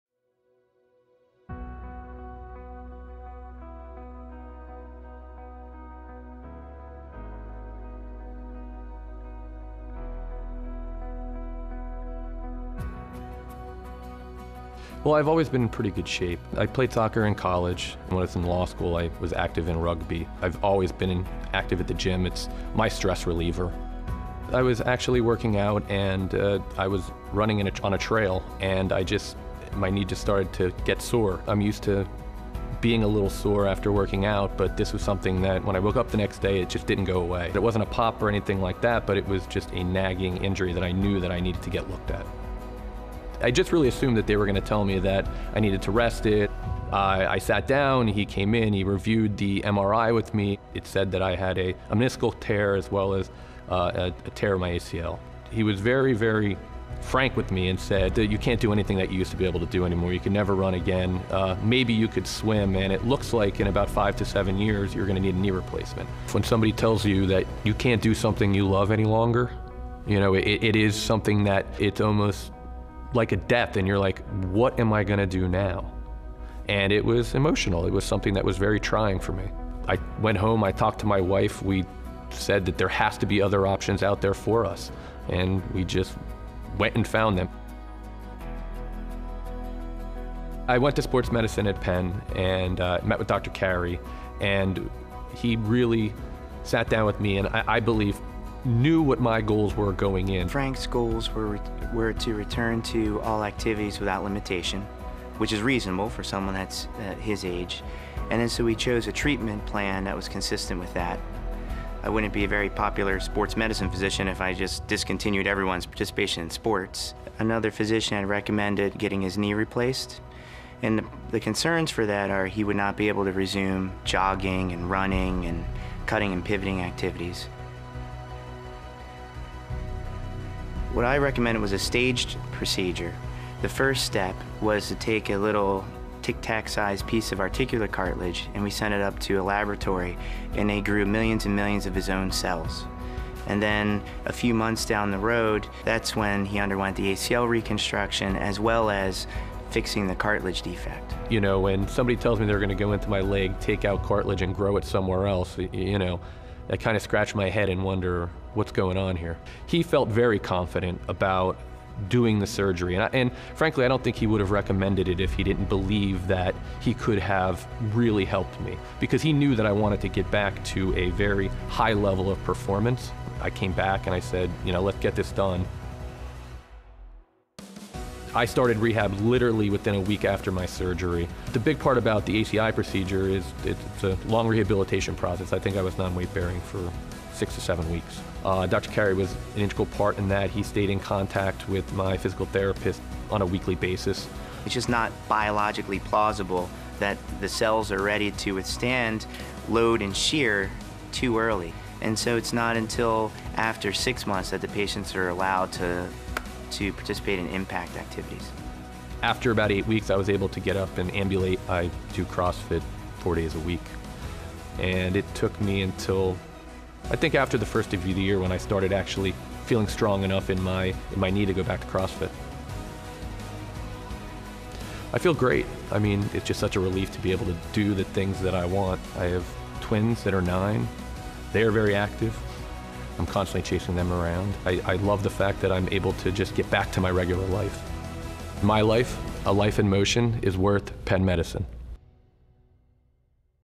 Cartilage Regeneration | Frank's Story
Frank had a a torn ACL and a meniscus tear in his knee. The first doctor he saw told him he needed a knee replacement, and would eventually need to stop his very active lifestyle. Frank wasn't ready for that. Instead, he came to Penn Sports Medicine for a second opinion. He met James Carey, MD, who told him about a procedure called autologous chondrocyte implantation (ACI).

In ACI, a tiny piece of cartilage was taken from Frank's knee and sent to a lab in which it grew millions of cells. Once it was ready, Dr. Carey repaired Frank's ACL tear, and re-implanted his knee cartilage. Today, Frank is back to his healthy lifestyle working out at his CrossFit gym and running.

Meet James Carey, MD, Director of the Penn Center for Cartilage Repair